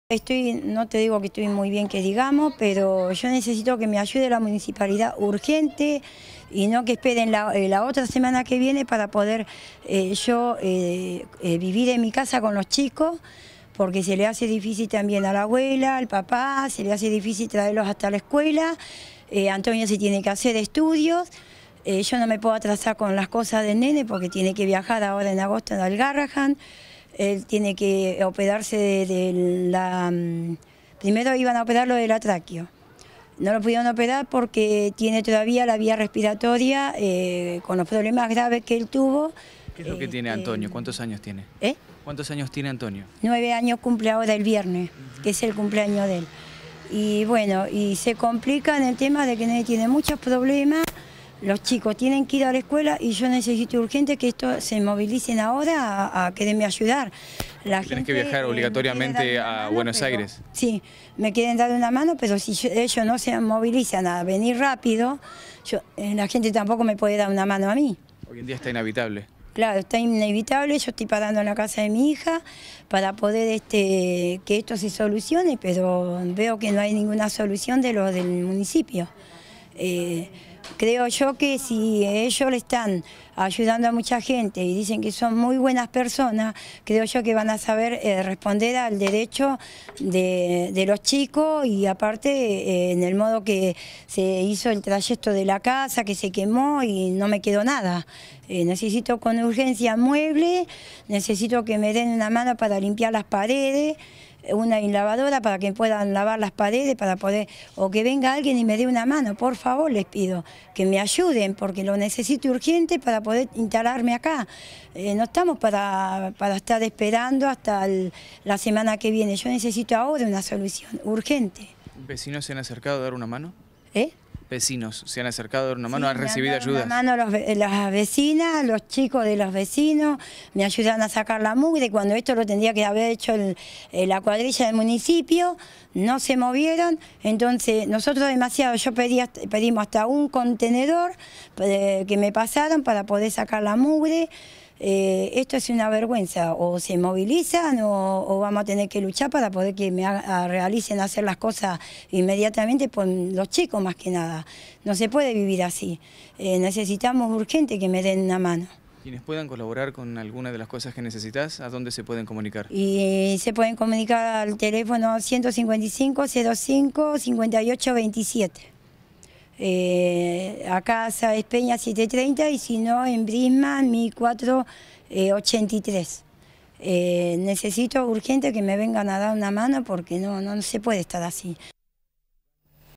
Ayuda urgente a una familia que perdió todo
Claudia Sepúlveda quiere volver a su casa de Saenz Peña 730  tras un incendio que provocó destrozos.